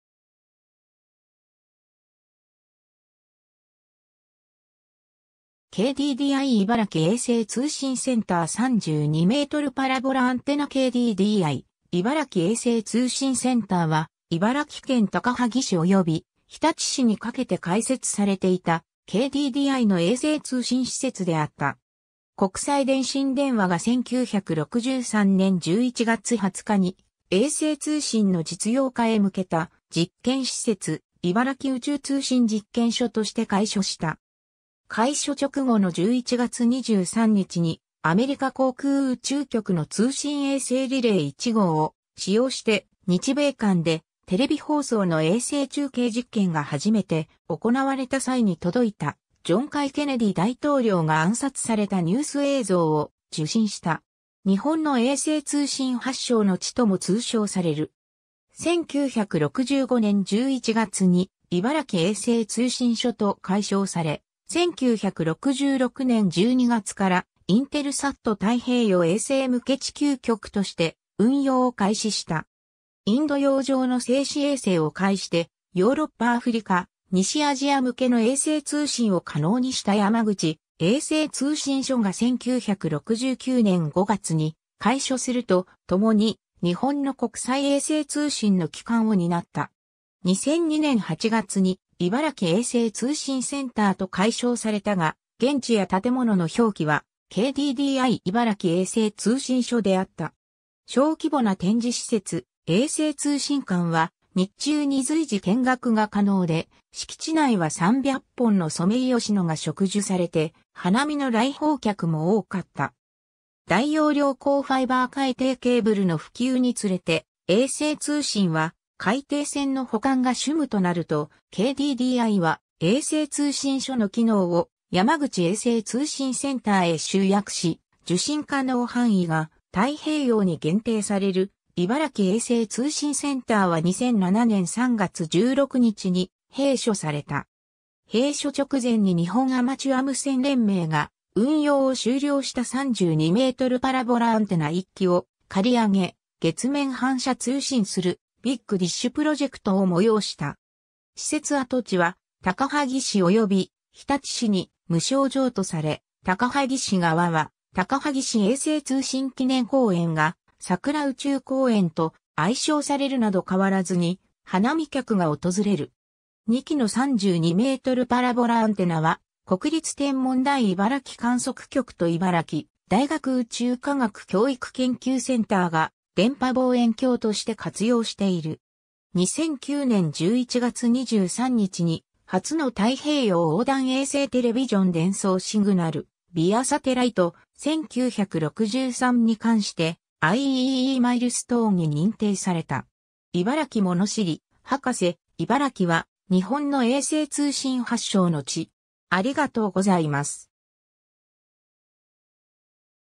KDDI茨城衛星通信センター
KDDI茨城衛星通信センター 32mパラボラアンテナ（2005年4月） KDDI茨城衛星通信センター（ケイディディアイ いばらきえいせいつうしんセンター）は、茨城県高萩市および日立市にかけて開設されていた、KDDIの衛星通信施設であった。
 国際電信電話 (KDD) が1963年（昭和37年）11月20日に、衛星通信の実用化へ向けた実験施設「茨城宇宙通信実験所」として開所した。
開所直後の11月23日に、アメリカ航空宇宙局 (NASA) の通信衛星リレー1号を使用して日米間でテレビ放送の衛星中継実験が初めて行われた際に届いた、ジョン・F・ケネディ大統領が暗殺されたニュース映像を受信した。
「日本の衛星通信発祥の地」とも通称される。
 1965年11月に「茨城衛星通信所」と改称され、1966年12月からインテルサット太平洋衛星向け地球局として運用を開始した。
インド洋上の静止衛星を介してヨーロッパ・アフリカ・西アジア向けの衛星通信を可能にした山口衛星通信所が1969年5月に開所すると、共に日本の国際衛星通信の基幹を担った。
2002年8月に「茨城衛星通信センター」と改称されたが、現地や建物の表記は「KDDI茨城衛星通信所」であった。
 小規模な展示施設「衛星通信館」は日中に随時見学が可能で、敷地内は300本のソメイヨシノが植樹されて花見の来訪客も多かった。
 大容量光ファイバー海底ケーブルの普及につれて衛星通信は海底線の補完が主務となると、KDDIは衛星通信所の機能を山口衛星通信センターへ集約し、受信可能範囲が太平洋に限定される茨城衛星通信センターは2007年（平成19年）3月16日に閉所された。
 閉所直前に日本アマチュア無線連盟が、運用を終了した32メートル (m) パラボラアンテナ1基を借り上げ、月面反射通信する「ビッグ・ディッシュ・プロジェクト」を催した。
 施設跡地は高萩市および日立市に無償譲渡され、高萩市側は高萩市衛星通信記念公園がさくら宇宙公園と愛称されるなど変わらずに花見客が訪れる。
2基の32mパラボラアンテナは、国立天文台茨城観測局と茨城大学宇宙科学教育研究センターが電波望遠鏡として活用している。
 2009年11月23日に「初の太平洋横断衛星テレビジョン伝送」 (First Transpacific Reception of a Television (TV) Signal via Satellite, 1963) に関してIEEEマイルストーンに認定された。
 いばらきもの知り博士:茨城は日本の衛星通信発祥の地